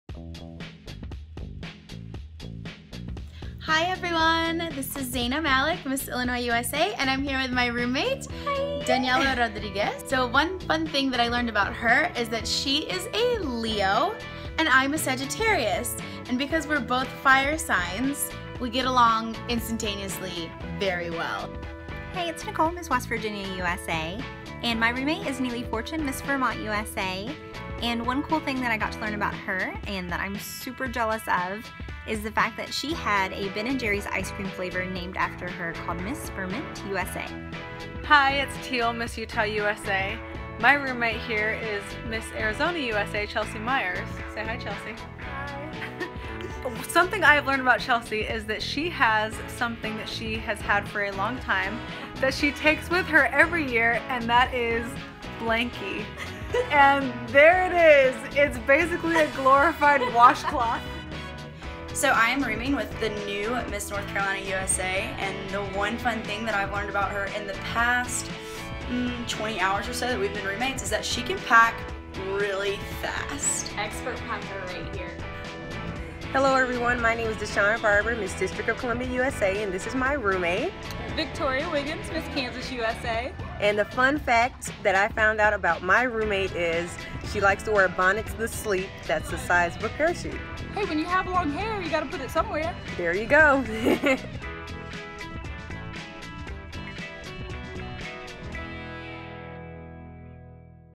2016 MISS USA Roomies Fun Facts
The contestants share one thing they have learned about their roommates while on-site for 2016 MISS USA!
Featuring: Miss Illinois USA 2016, Zena Malak; Miss West Virginia USA 2016, Nichole Greene; Miss Utah USA 2016, Teale Murdock; Miss Alabama USA 2016, Peyton Brown;  Miss DC USA 2016, Deshauna Barber